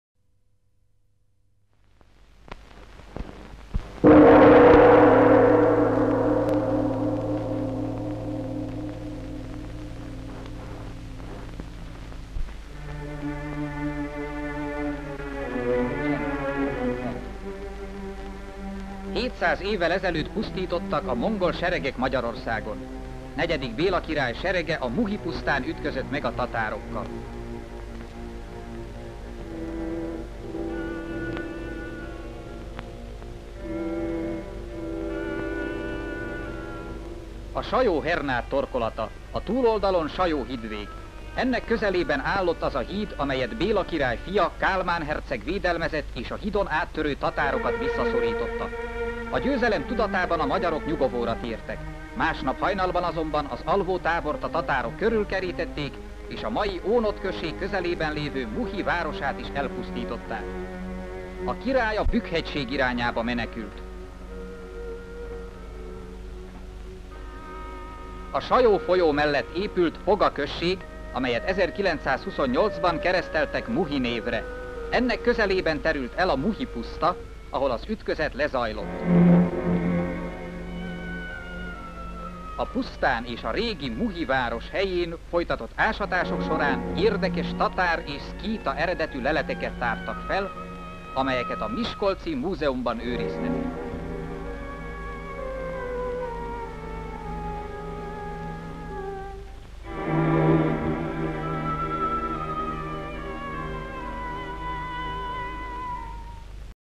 Hétszáz évvel ezelőtt pusztítottak a mongol seregek Magyarországon. (1941)
Hétszáz évvel ezelőtt pusztítottak a mongol seregek Magyarországon. IV. Béla király serege a Muhi-pusztán ütközött meg a tatárokkal. A Sajó-Hernád torkolata. A túloldalon Sajóhídvég. Ennek közelében állott az a híd, amelyet Béla király fia, Kálmán herceg védelmezett, és a hídon áttörő tatárokat visszaszorította. A győzelem tudatában a magyarok nyugovóra tértek. Másnap hajnalban azonban az alvó tábort a tatárok körülkerítették, és a mai Ónod község közelében levő Muhi városát is elpusztították. A király a Bükk-hegység irányába menekült. A Sajó folyó mellett épült Kova község, amelyet 1928-ban kereszteltek Muhi névre. Ennek közelében terül el a Muhi-puszta, ahol az ütközet lezajlott. A pusztán és a régi Muhi város helyén folytatott ásatások során érdekes tatár és szkíta eredetű leleteket tártak fel, amelyeket a Miskolci Múzeumban őriznek.